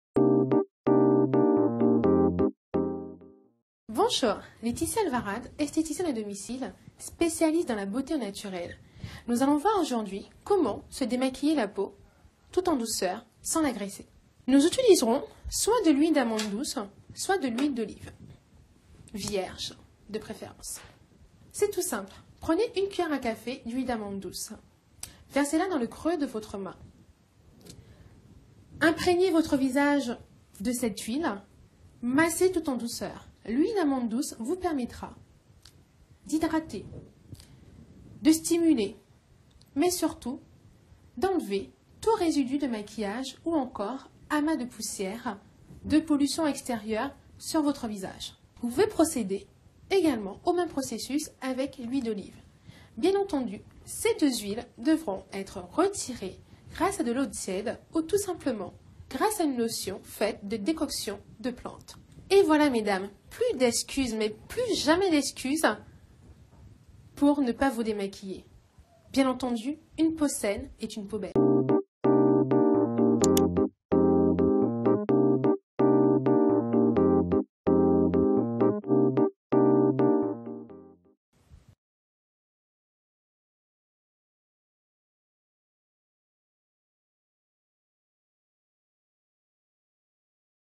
Comment se démaquiller en douceur
Comment se démaquiller naturellement en douceur ? Si vous ne savez pas quel produit démaquillant naturel est vraiment efficace alors suivez ces explications.

Laetitia Alvarade, esthéticienne et spécialiste de la beauté au naturel (rstyle.fr), vous propose une astuce efficace de démaquillage naturel.